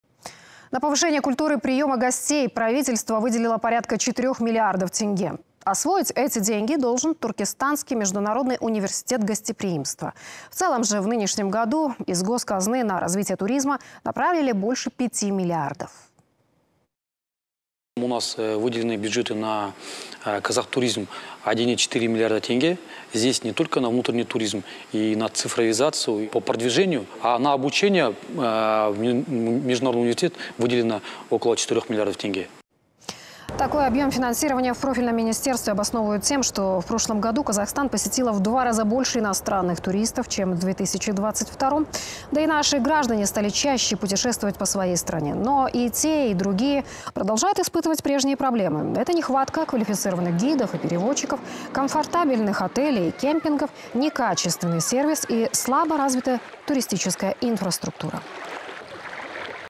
На повышение уровня гостеприимства выделено 4 миллиарда тенге
На повышение культуры приема гостей правительство выделило порядка 4 миллиардов тенге. Освоить эти деньги должен туркестанский международный университет гостеприимства. В целом же в нынешнем году из госказны на развитие туризма направили больше 5 миллиардов.